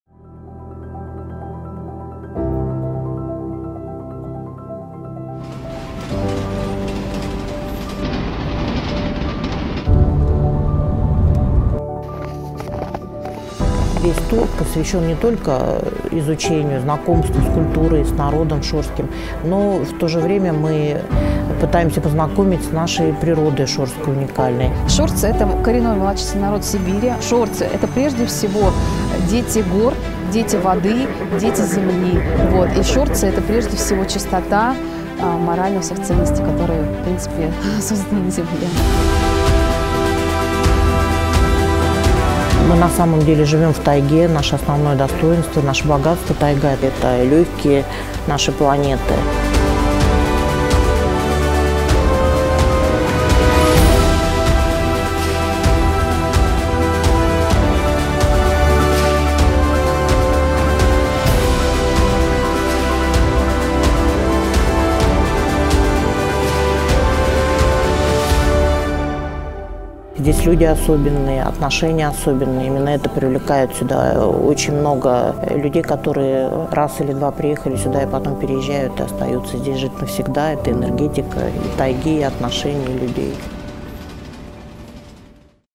Горная Шория. К детям тайги, Кемеровская область/Mountain Shoria, Kemerovo region
Горная Шория. К детям тайги, Кемеровская область / Mountain Shoria. To the children of taiga, Kemerovo region

Данное видео представлено на Всероссийский туристский видеофестиваль "ДИВО РОССИИ" / This video is presented at the All-Russian tourist video festival "DIVO OF RUSSIA"

Авторы - ООО "Эгида" совместно с АСИ

Номинация - Этнический туризм

ВНИМАНИЕ!!!